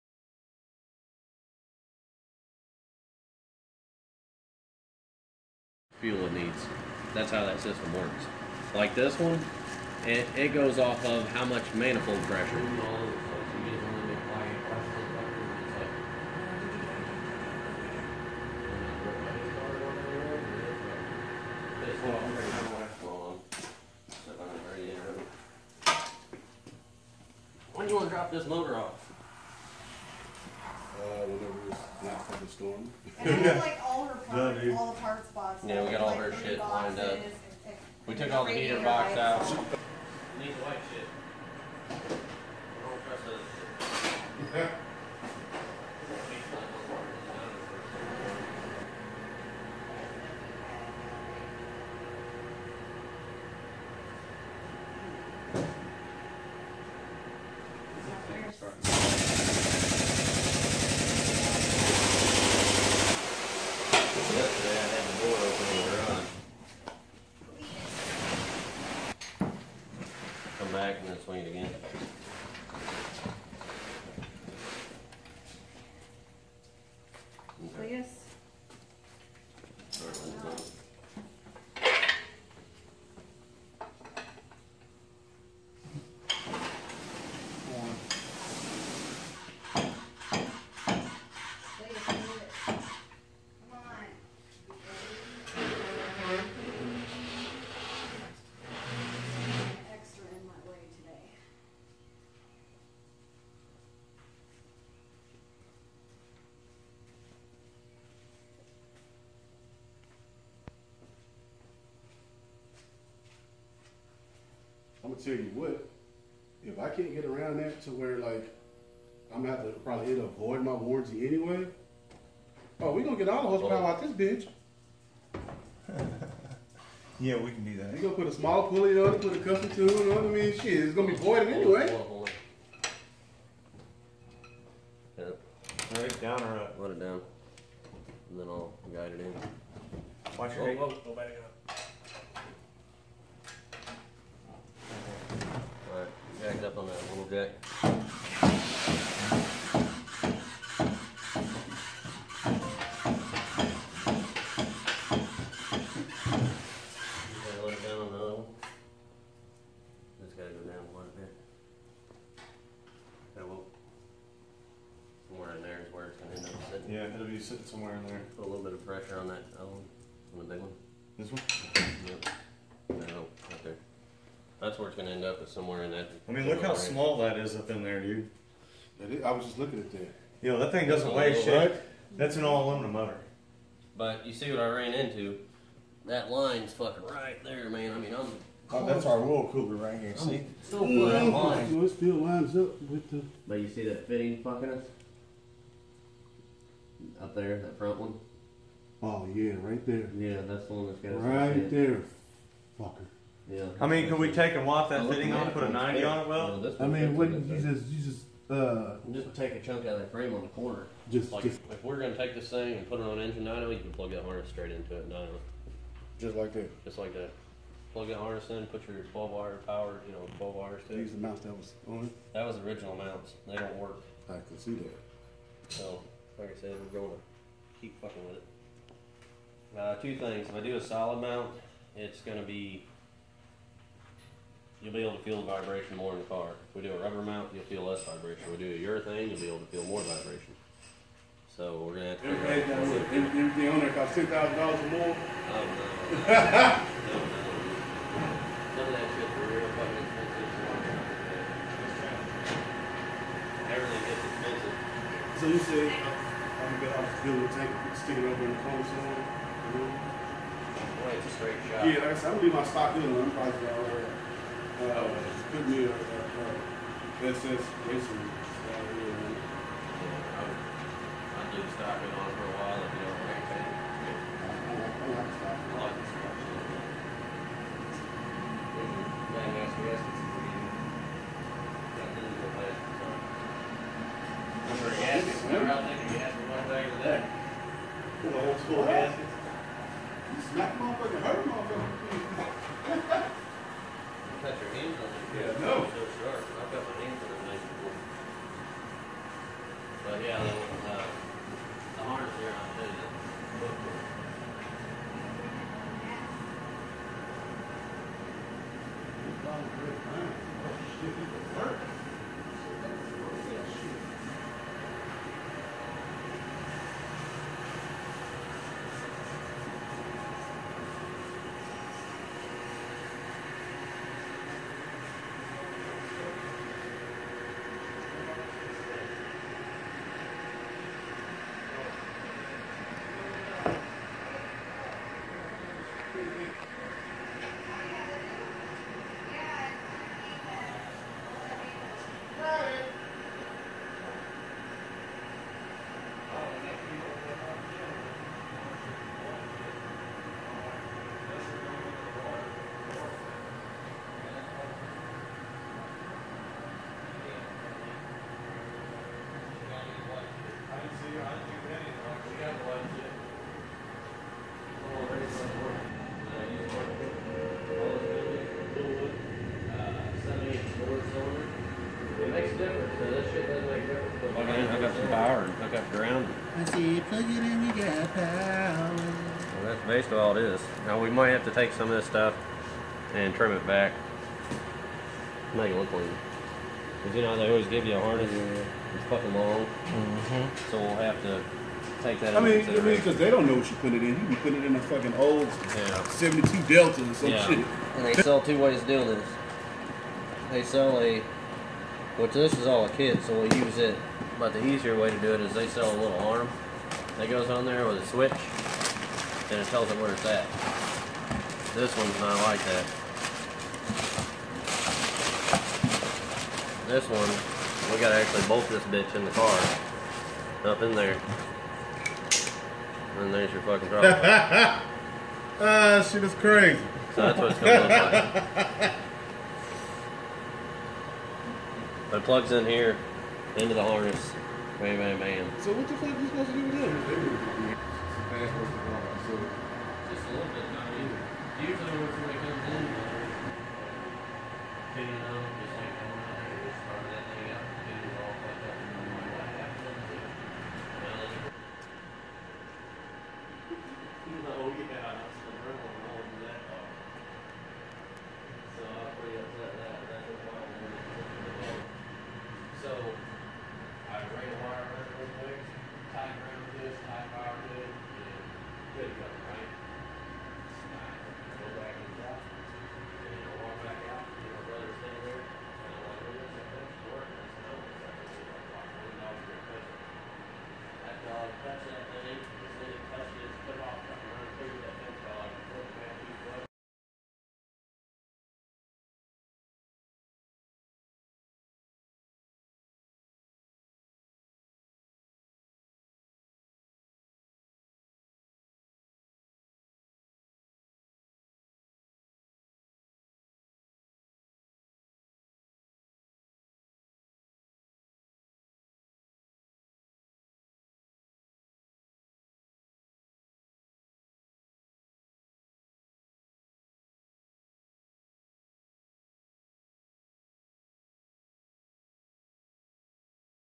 1st In The World 2016 Z-06 Supercharged LT4 Crate Engine Install in a 1978 Malibu
2016 Supercharged LT4 Crate Engine Install in a 1978 Malibu